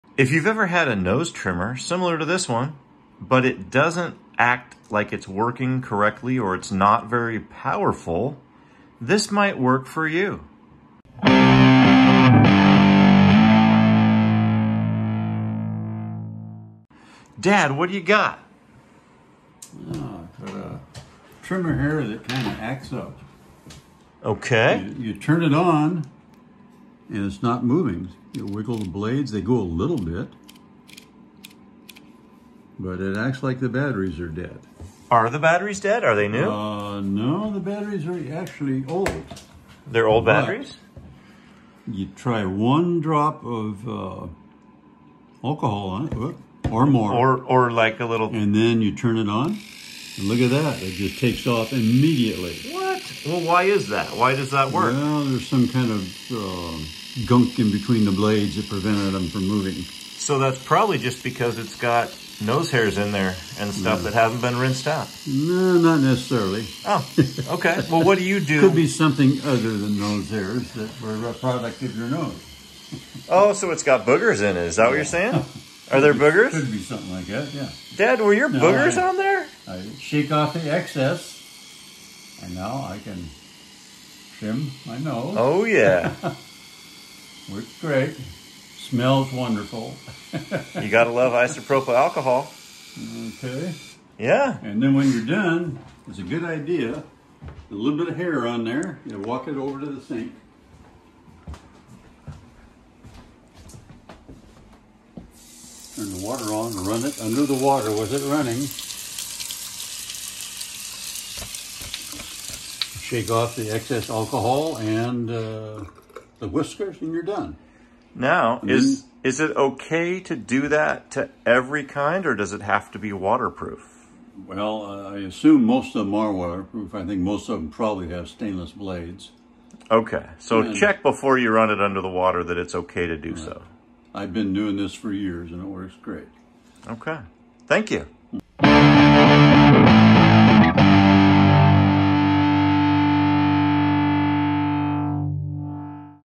Fix Nose Hair Trimmer- Make it run like new!!
Does your nose hair trimmer bog down and act like it has weak batteries? Try this fix and let me know if it works for you! This model is a Microtouch Switchblade, but this trick works on all different makes and styles.